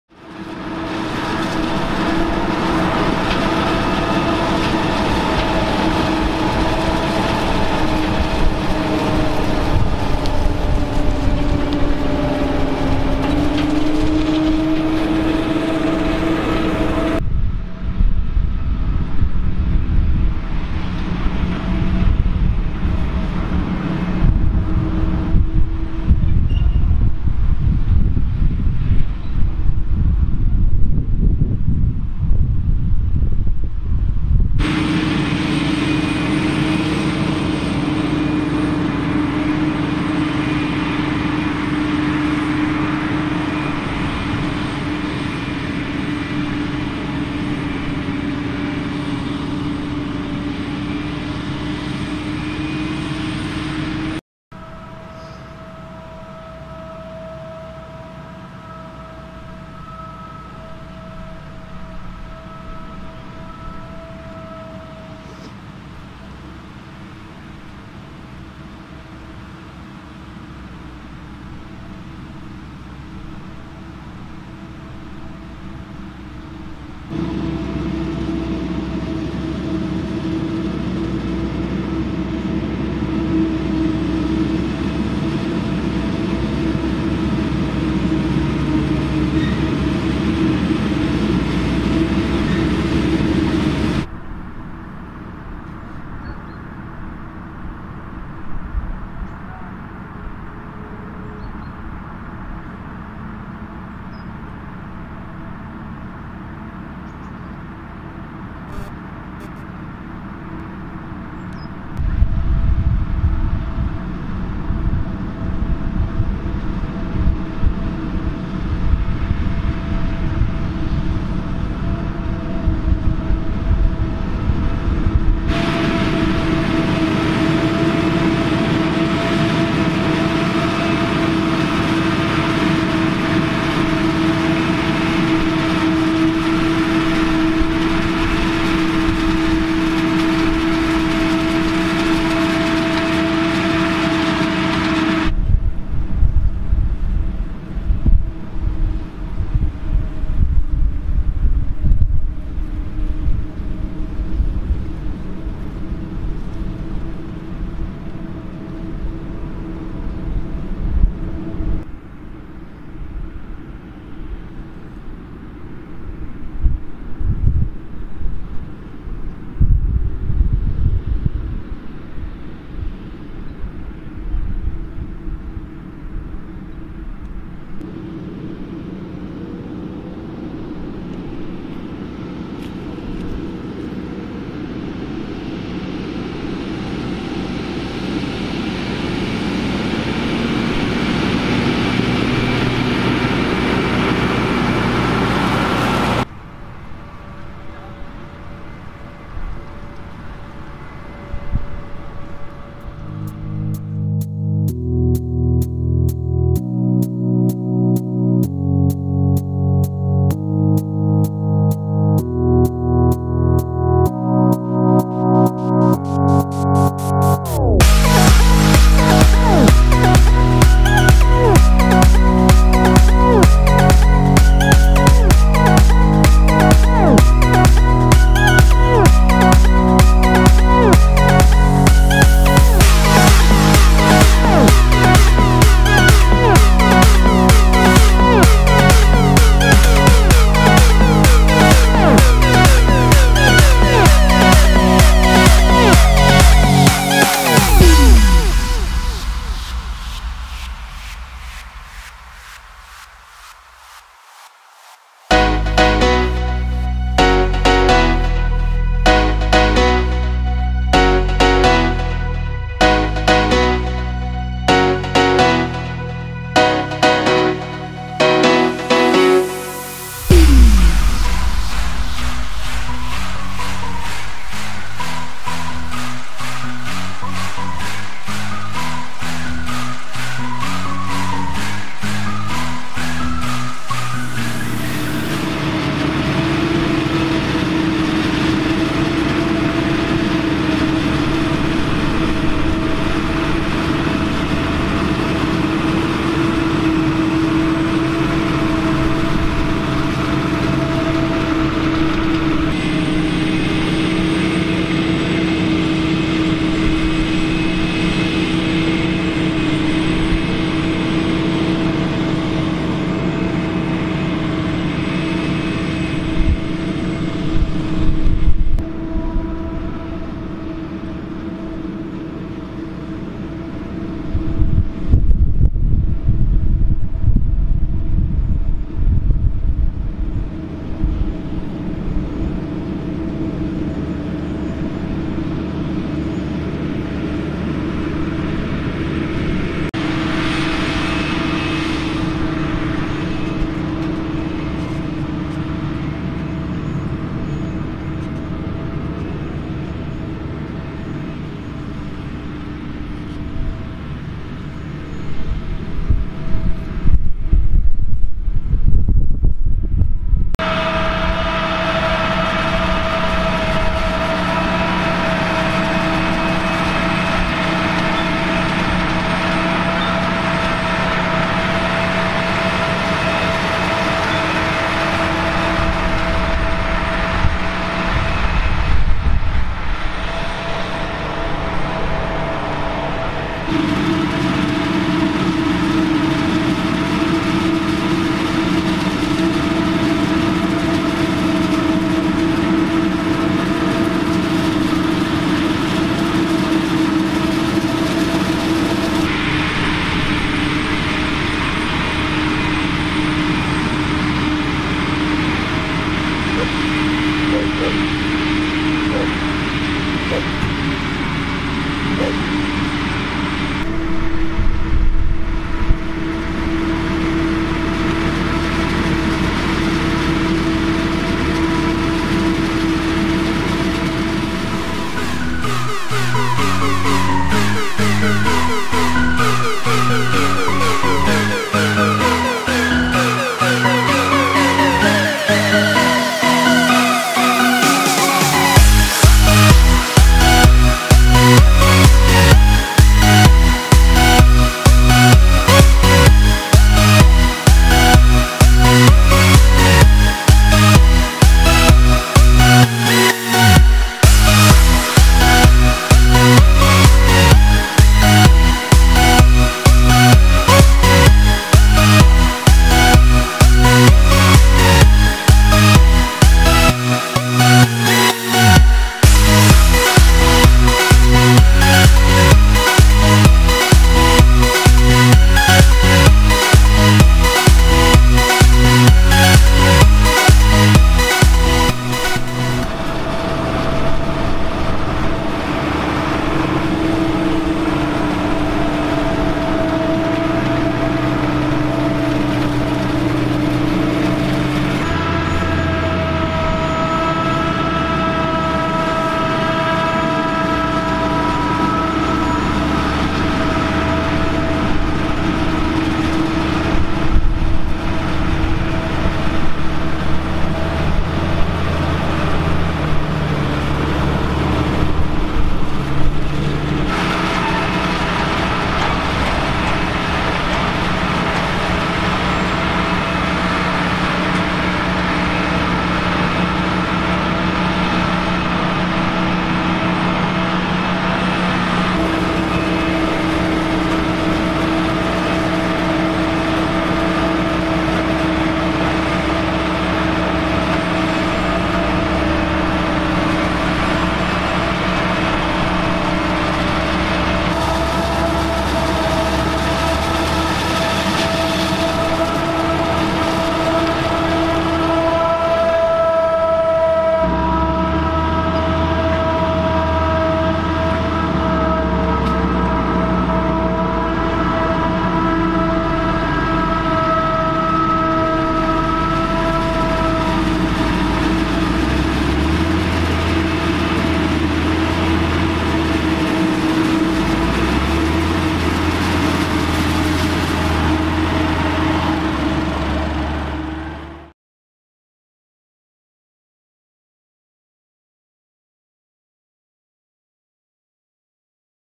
Mais hakselen 2019 | Kappelhof | John Deere 8500 i | Fendt 515 C + 720 + 828 l Massey Ferguson 7620
✔ Werkzaamheden: Mais hakselen | Mais häckseln | Harvesting maize
✔ Loonbedrijf: Kappelhof
✔ Machines: John Deere 8500 i
✔ Plaats: Münsterland